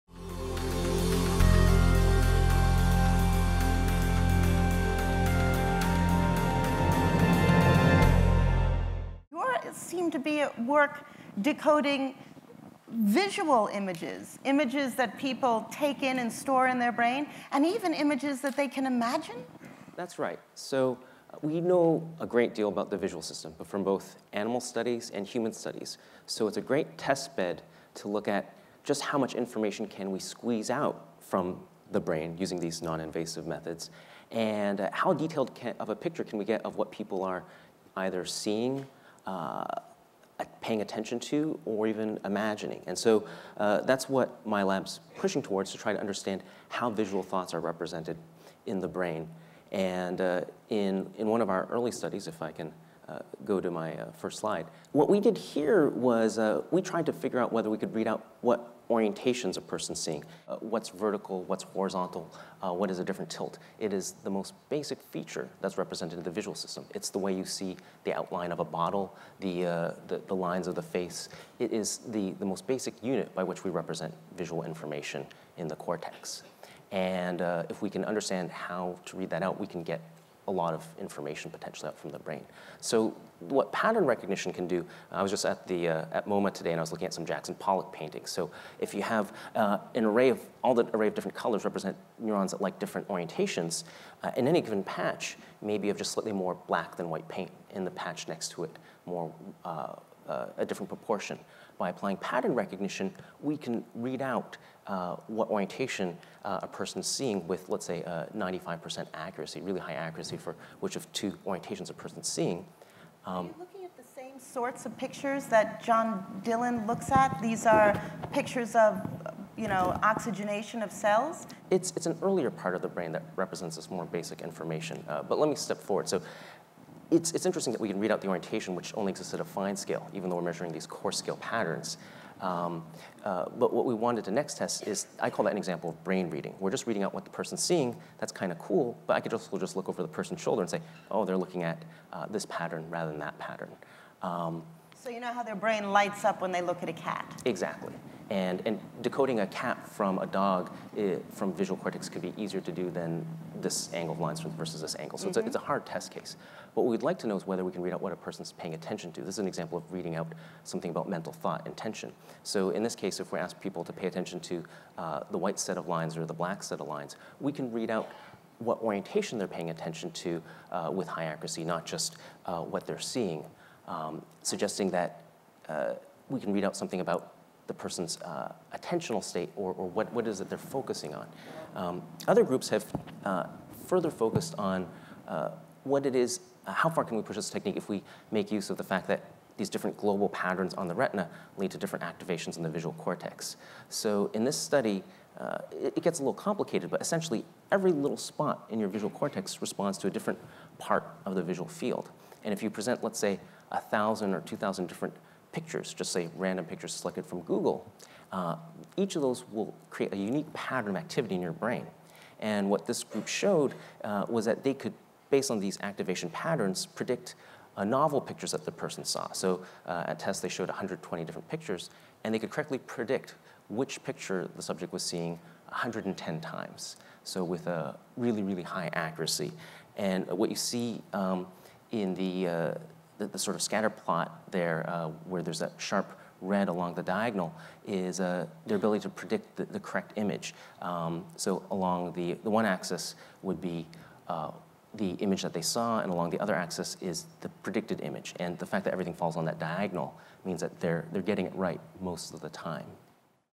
Decoding the Visual Field
Everything you see produces a unique pattern of activity in your brain. Can scientists studying this data predict what you are visualizing? Frank Tong explains the technology, and startling accuracy, of this scientific endeavor.